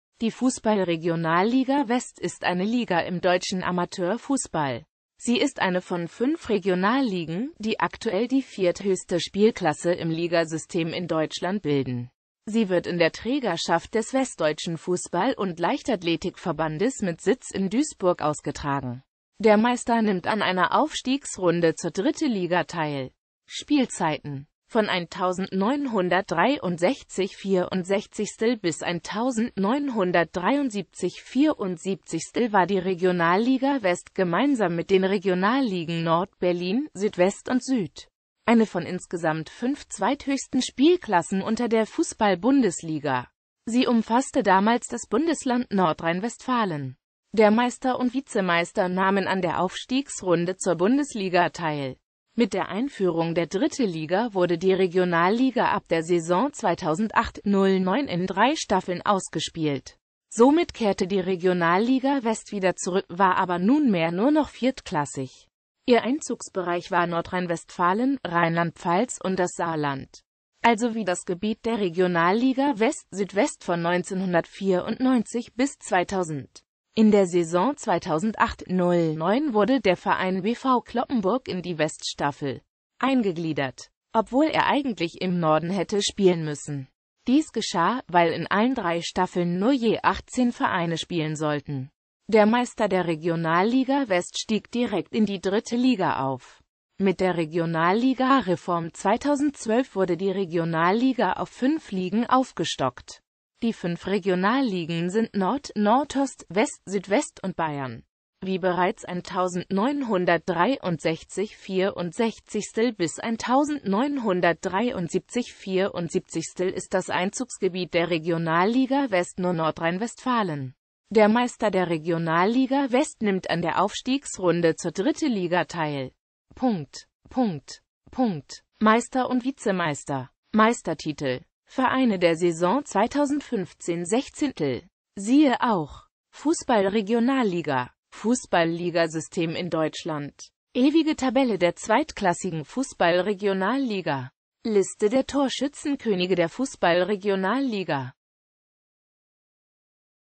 Fußball-Regionalliga West
Vielen Dank für Ihre Unterstützung:

Die Fußball-Regionalliga West ist eine Liga im deutschen Amateurfußball.Sie ist eine von fünf Regionalligen, die aktuell die vierthöchste Spielklasse im Ligasystem in Deutschland bilden.Sie wird in der Trägerschaft des Westdeutschen Fußball- und Leichtathletikverbandes (WFLV) mit Sitz in Duisburg ausgetragen.Der Meister nimmt an einer Aufstiegsrunde zur 3.

Bild-Copyright-Informationen
Urheber Info: Muns